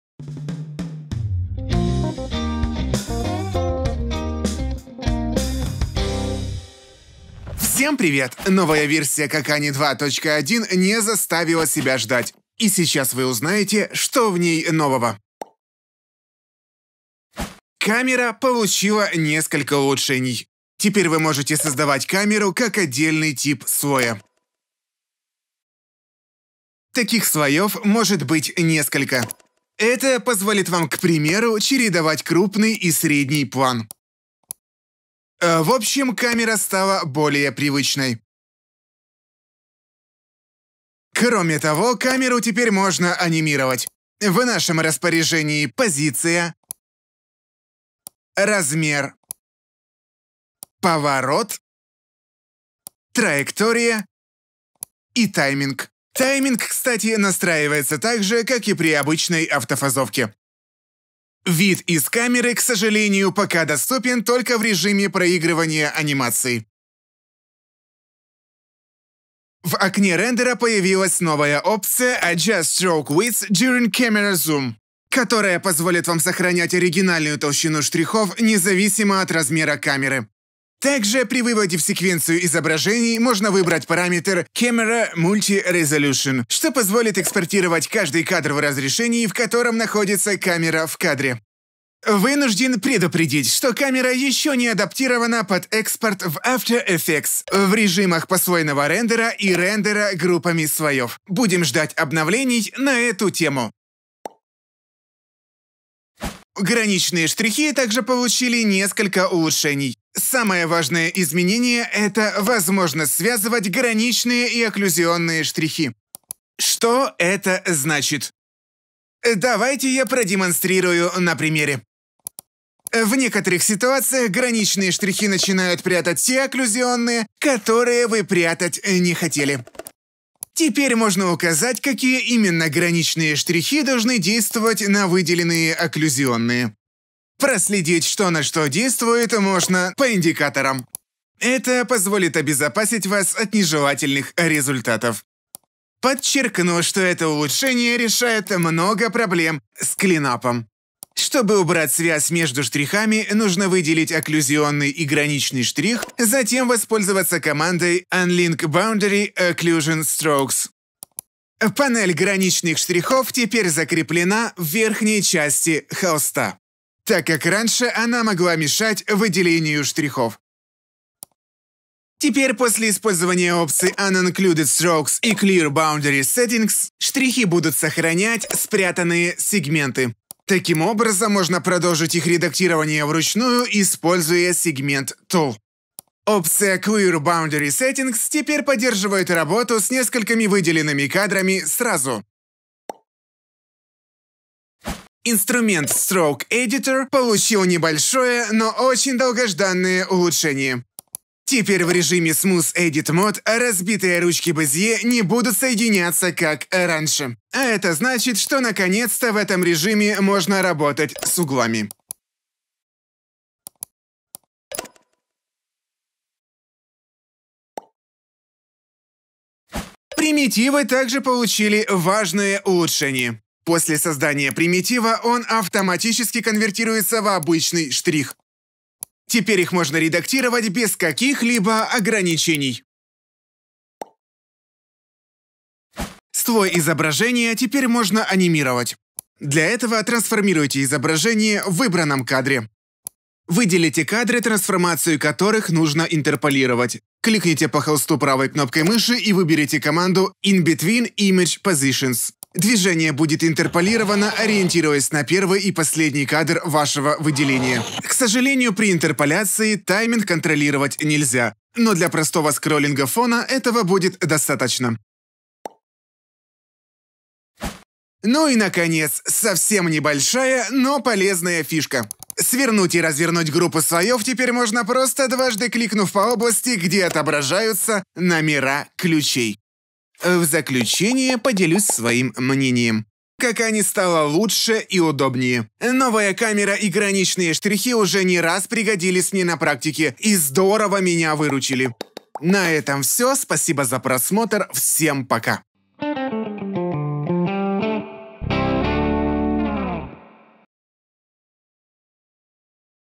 CACANi 2.1 Обзор новых возможностей
В этом видео обзоре вы узнаете, что нового в CACANi 2.1. CACANi это программа для 2д анимации с функцией автофазовки, которая значительно упрощает жизнь аниматорам.